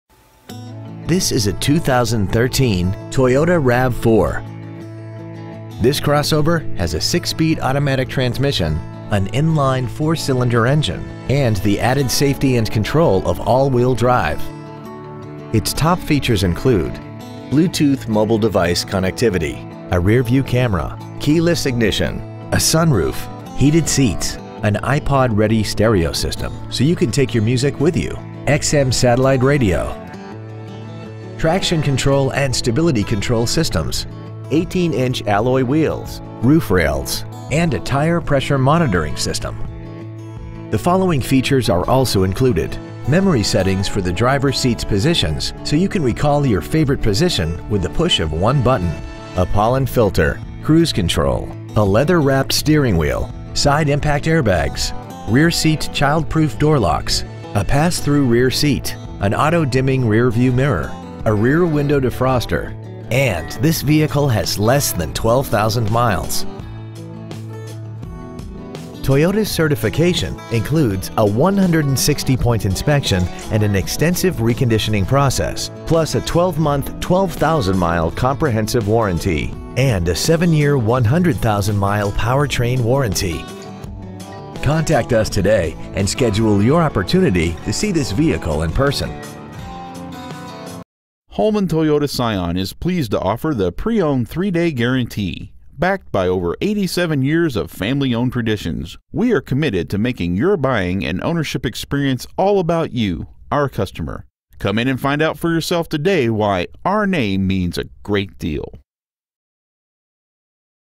2013 Toyota RAV4 Certified Mount Laurel NJ 08054
We've been honored to serve the Mount Laurel NJ area , we promise that your experience at our dealership will exceed your expectations ! Year : 2013 Make : Toyota Model : RAV4 Engine : Gas I4 2.5L/152 Trans . : Automatic Exterior : Black Miles : 11,034 Interior : Terracotta Stock : DW123476 Holman Toyota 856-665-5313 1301 Route 73 N Mount Laurel , NJ 08054